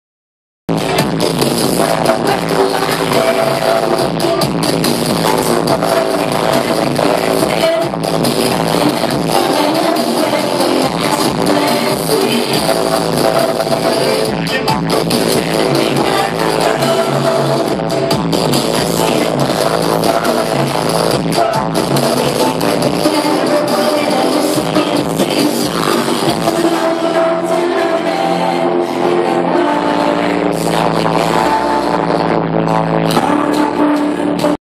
Deadmau5 Show - Tommy Lee & DJ Aero - Austin, TX September 14, 2011
Just a short clip, was making sure battery life would last for Deadmau5

Recorded on EVO 3D